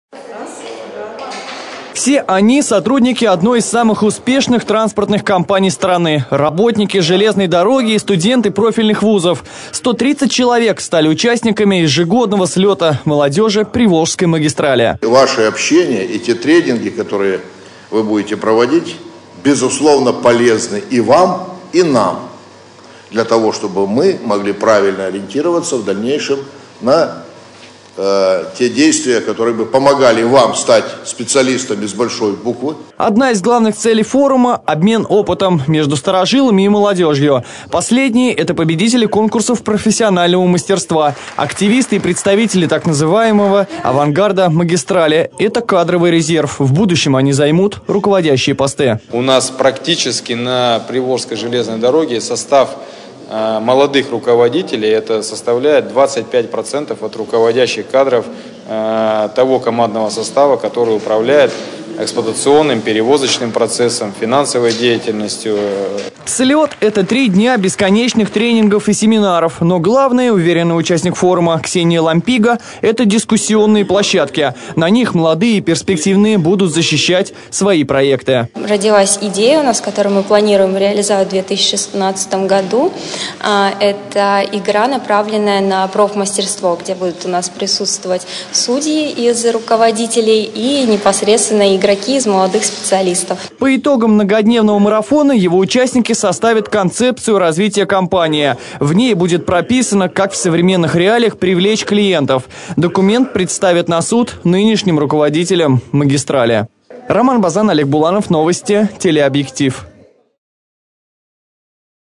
В Саратове стартовал слет молодых железнодорожников.
Три дня сотрудники магистрали и студенты профильных вузов, которым еще нет тридцати, будут создавать новую концепцию. Решать, как в современных условиях угодить клиентам...